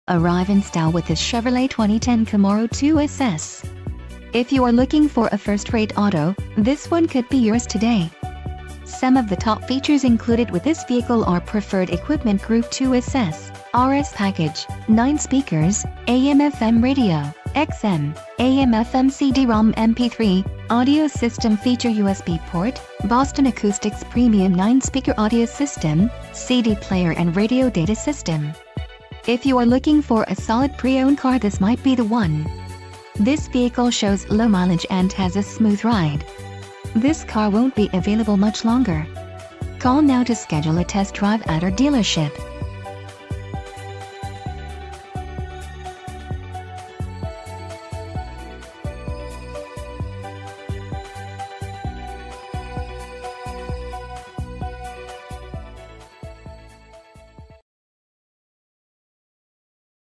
2010 Chevrolet Camaro 2SS
Recent Arrival! CLEAN CARFAX, LEATHER, SUNROOF, 20 x 8 Front  and  20 x 9 Rear Aluminum Wheels, Bluetooth For Phone, Body-Color Roof Ditch Molding, Boston Acoustics Premium 9-Speaker Audio System, Garage door transmitter, Heated Driver  and  Front Passenger Seats, High-Intensity Discharge Headlamps, Power driver seat, Rear Parking Sensors, Remote keyless entry, RS Package, RS Unique Tail Lamps, Speed control, Universal Home Remote. RWD 6-Speed Manual 6.2L V8 SFIAwards:* 2010 KBB.com Brand Image AwardsNobody beats a Veterans Ford deal!!